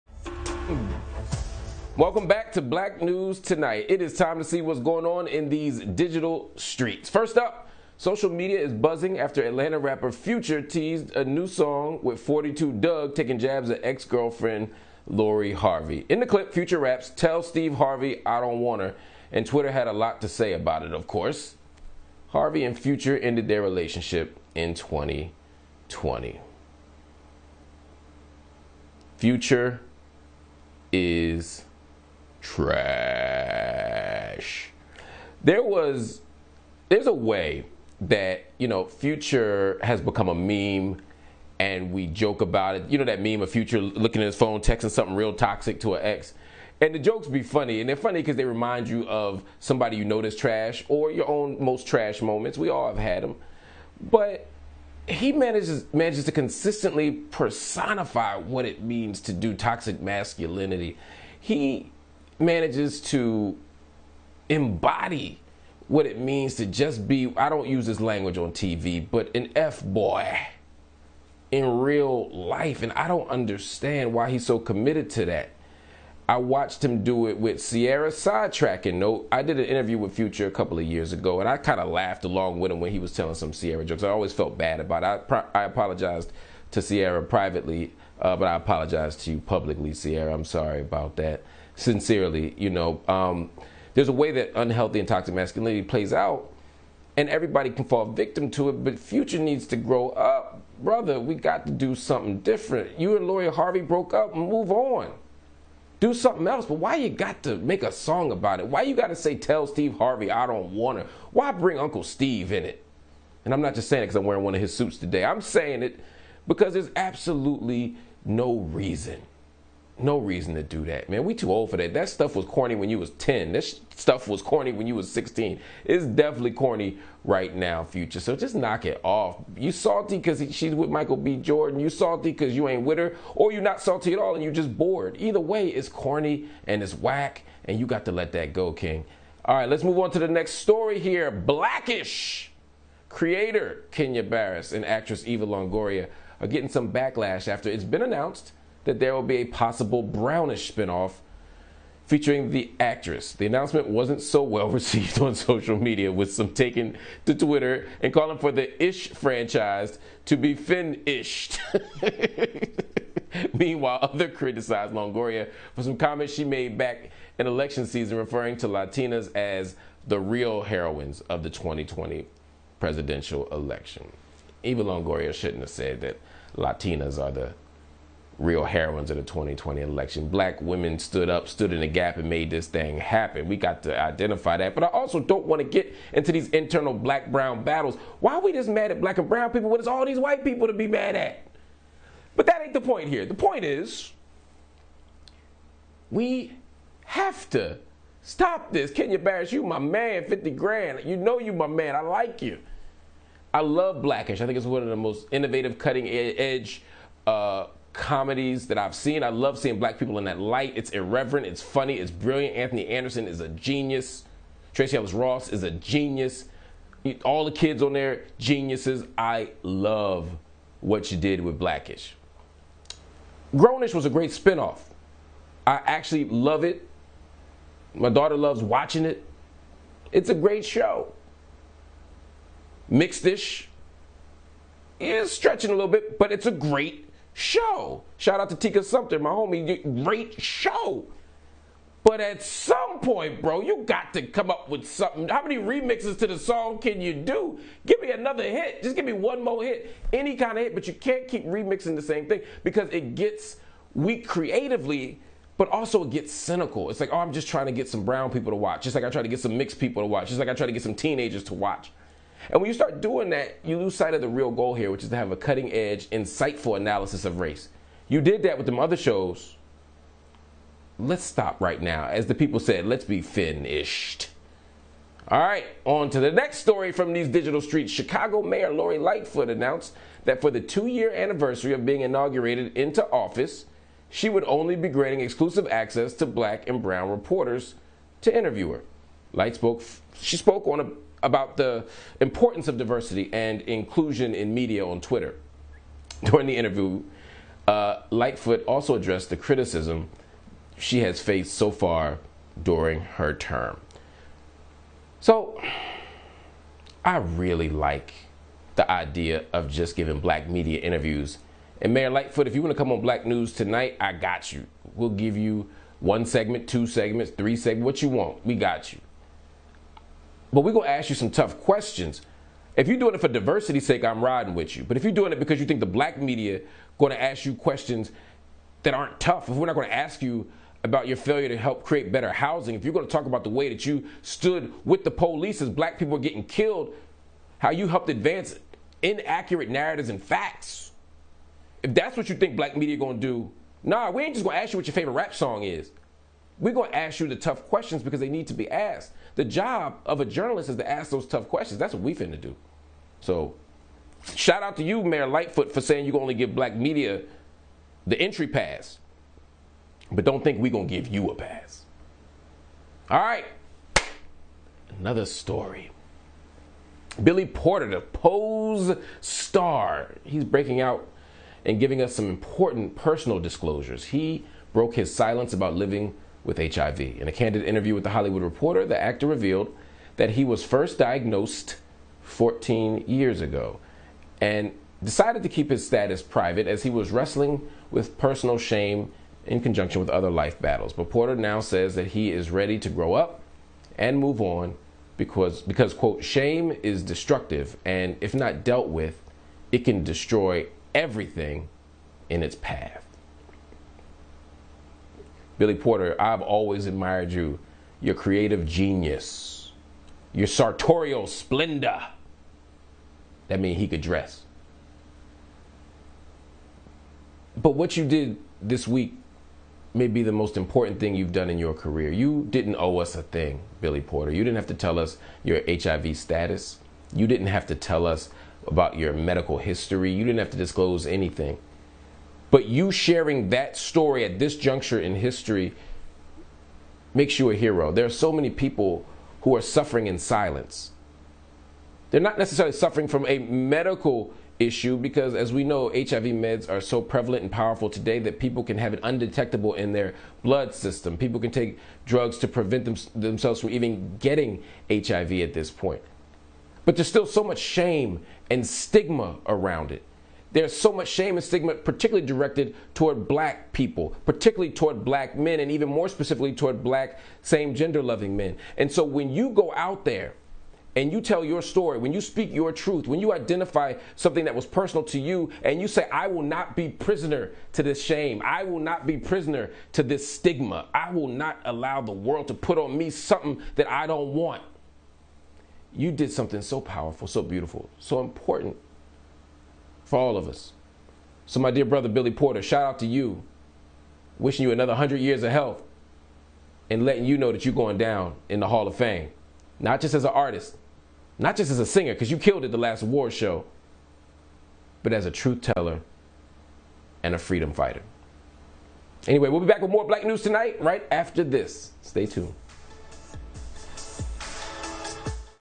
Marc Lamont Hill: ‘Future is Trash’ for Lori Harvey Dig
Marc Lamont Hill covers what's going on in "these digital streets." First, he covers Future's new song when he takes a dig at Steeve Harvey's daughter, Lori Harvey.

"Future is trash," Hill says of the rapper. "He manages to consistently personify what it means to do toxic masculinity."

Hill also talks about the backlash against "Black-ish" creator Kenya Barris over a possible series with Eva Longoria called "Brown-ish."

"You can't keep remixing the same thing," Hill says. "It gets weak creatively, but also it gets cynical."